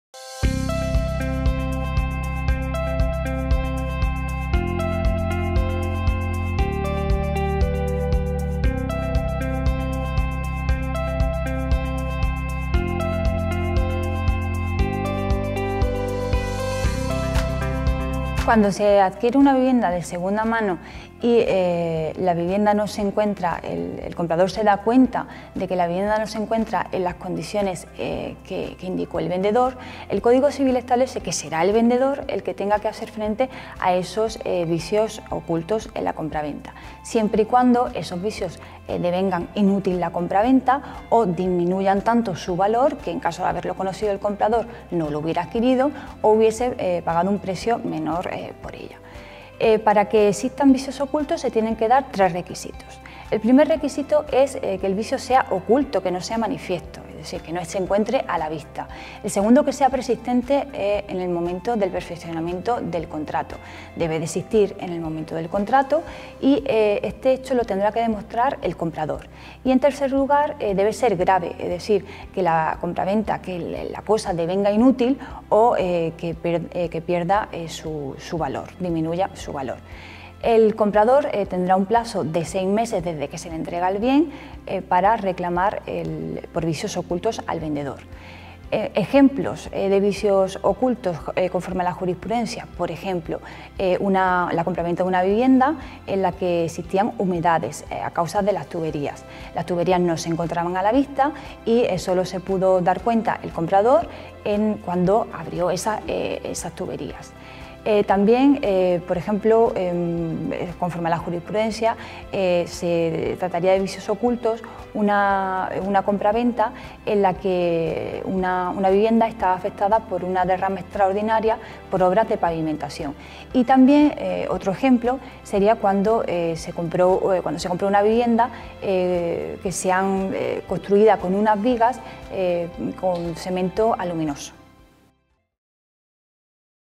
¿Qué son los vicios ocultos de una vivienda?
¿Qué  son  los  vicios  ocultos  de una  vivienda?  La  abogada  Irene  Delgado,  abogada  especialista  en el  sector  inmobiliario  nos  resuelve  las  dudas  que  se  plantean  con  los  vicios  ocultos  que  aparecen  al  adquirir  una  vivienda  de segunda  mano.  Desde  la  fecha  dela  transacción,el  comprador  tiene  6  meses  para  reclamarle  al  vendedor  cualquier  vicio  oculto  o  desperfecto  que  estuviese  presente  en el momento  dela  compraventa  y  que  haga  disminuir  el  precio  del  inmueble  adquirido.